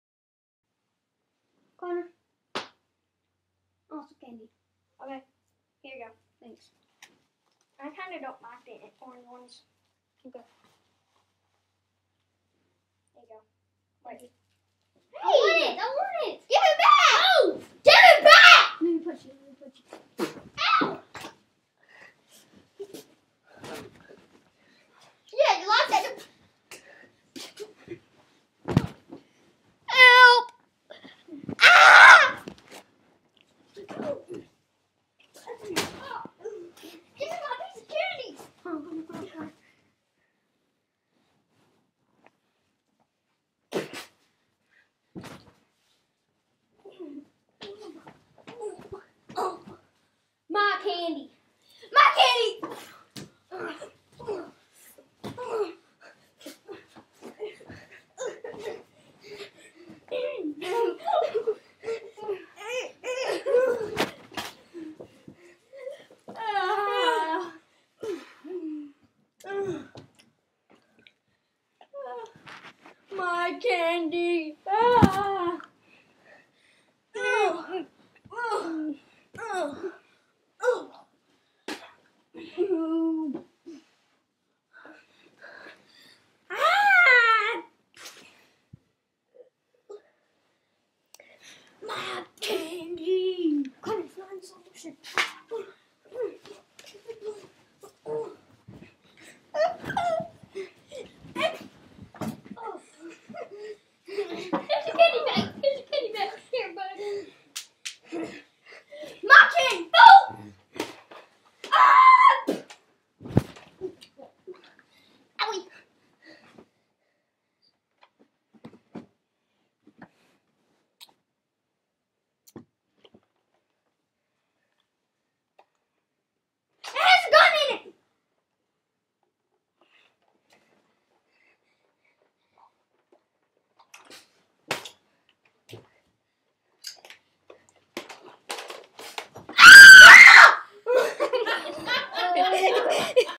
Play fighting
My brother Connor and I Play fighting.
Wait till you see what happens.